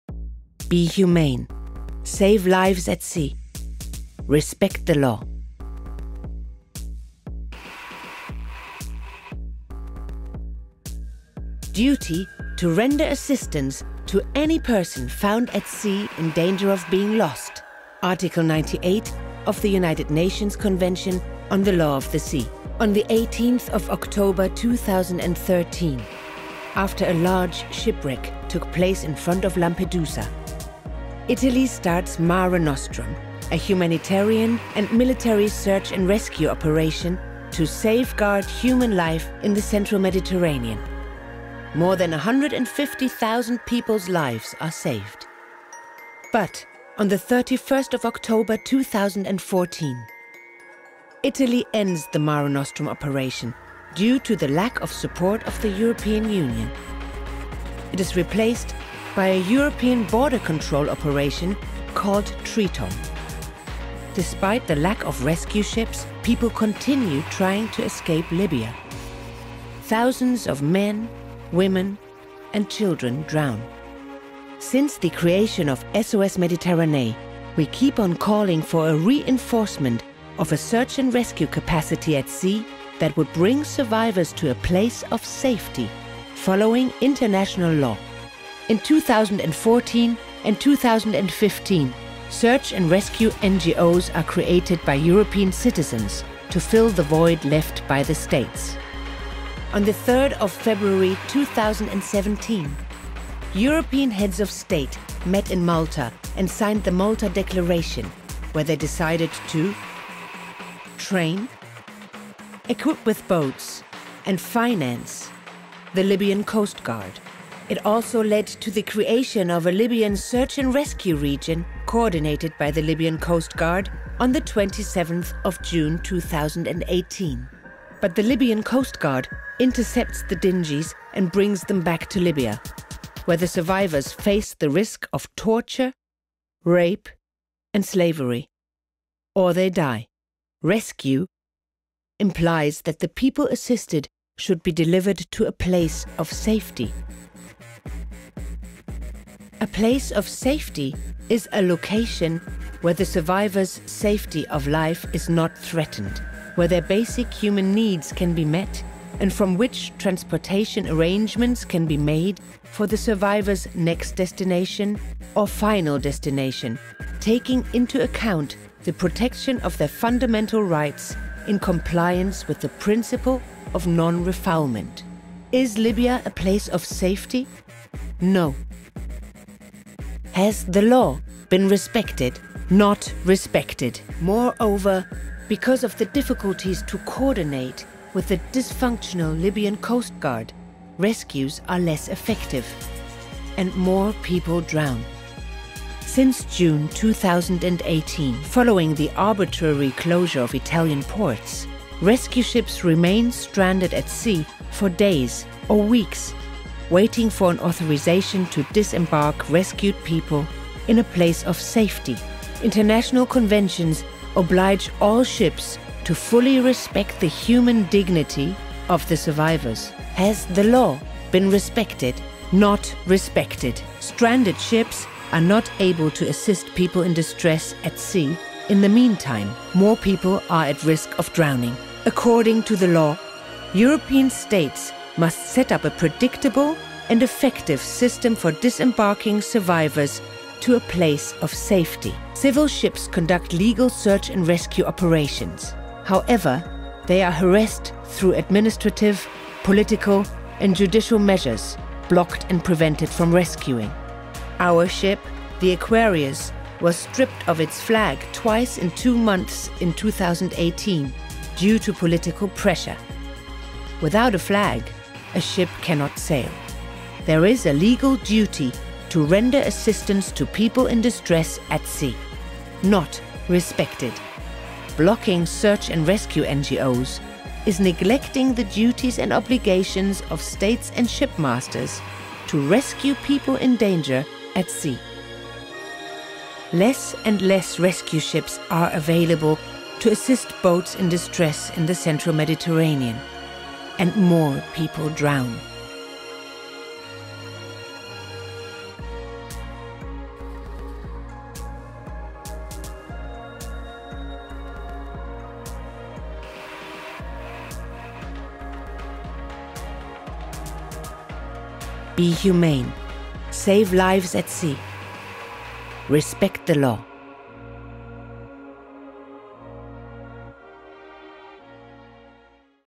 RespectLawOfTheSea - Summary
"Be humane, save lives at sea, respect the law!"

Since 2014 nearly 20,000 people have died trying to flee via the Mediterranean (IOM) - a humanitarian disaster that continues and could have been avoided if European states had employed enough means to save people from ditress at sea.

In this latest summarizing video of our series RespectLawOfTheSea, we take a look back at five years of deterioration and violations of international maritime and humanitarian law in the Central Mediterranean.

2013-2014: MARE NOSTRUM, the Italian maritime operation rescuing 150.000 people in the Mediterranean is suspended because of lack of support of other European states
Since February 2017: the Libyan Coast Guard is strengthened and a search and rescue area managed from Tripoli is created
Since 2017: search-and-rescue NGOs face several obstacles curtailing their life-saving missions
Since June 2018: European states close their ports for people rescued in the Mediterranean. Rescue ships are blocked at sea.

Find out more about our series RespectLawOfTheSea and visit our